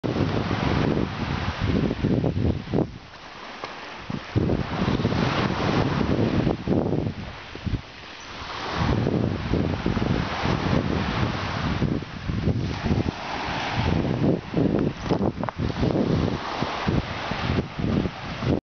Beach Riding
A short video of Timba and I last summer. :) Sorry about the sound of the wind,I live in Newfoundland,all we have is wind.